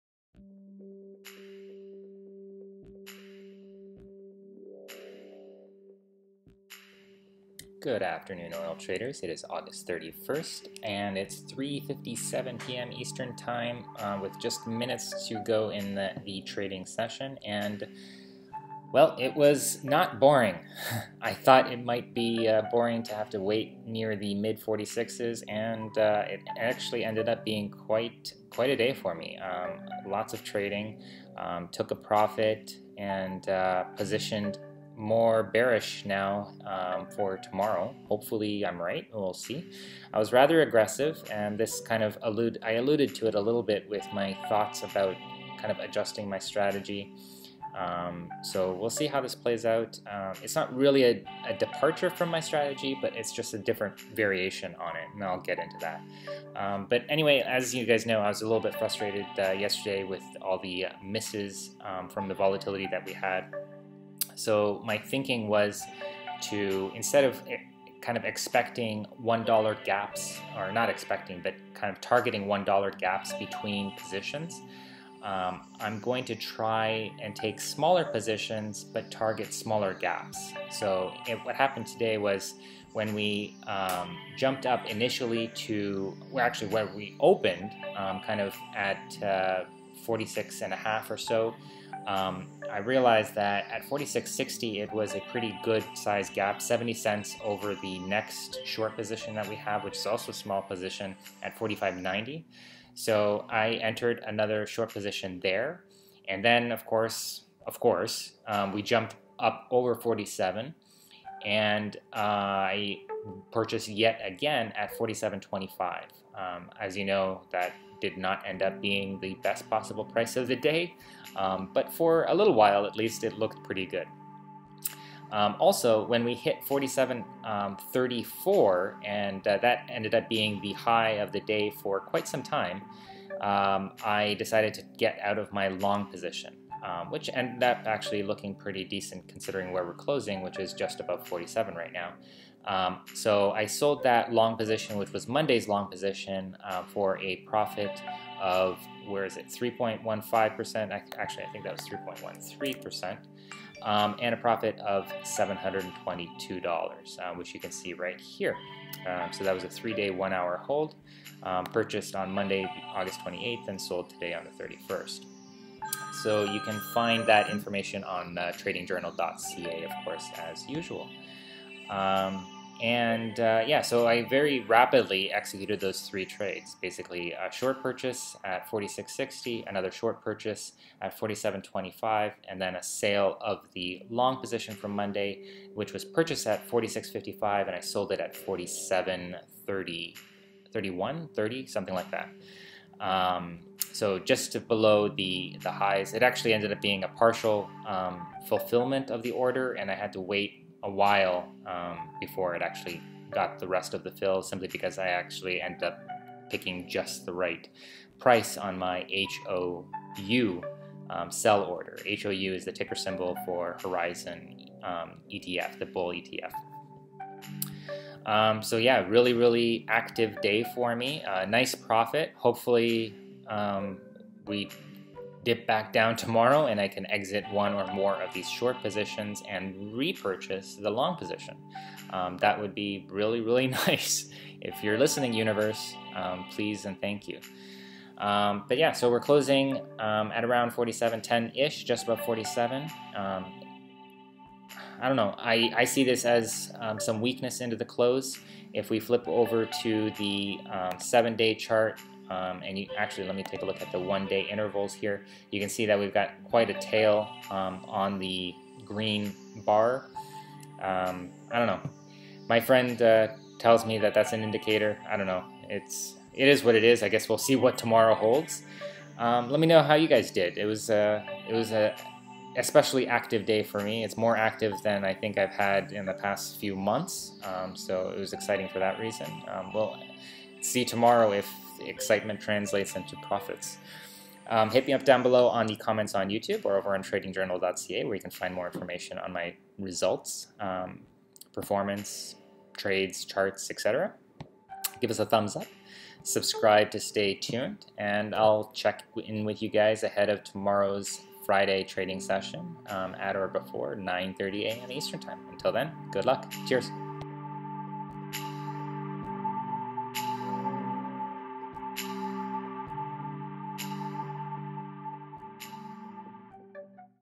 116 - Results for Aug 31, 2017 - $722 Profit on a Bull Run Above $47, Finally. Bought Short Twice!
* Yes, I was frustrated from yesterday’s misses, but I “don’t think” that’s why I was aggressive with buy play today
    * I bought short at $46.60 shortly after the open
        * It’s a 70-cent gap over my next short positioned at $45.90
        * I typically aim for a bout $1 gaps, but it’s a smaller buy
* We then jumped above $47 and I bought short again at $47.25
* When we hit $47.34, I entered my sell order for the $46.55 long and sold it at $47.30 for 3.13% ($722 Profit)
* It was looking like we were going higher still, but ultimately settled at $47.10-ish